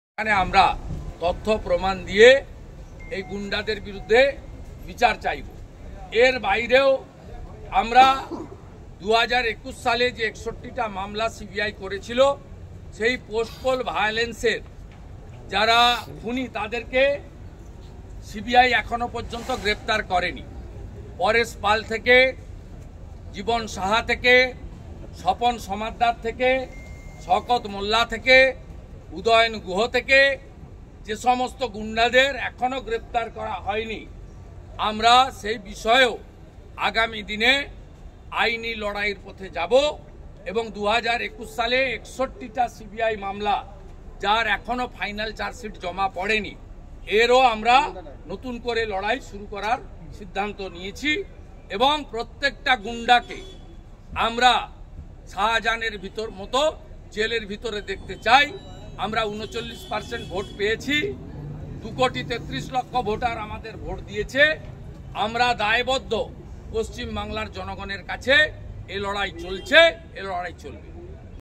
CBI কেন গ্রেফতার করছে না?
ভোটের পর রাজ্য জুড়ে সন্ত্রাস চলছে।রবিবার আক্রান্তদের নিয়ে রাজভবনে রাজ্যপালের সঙ্গে দেখা করেন শুভেন্দু অধিকারী।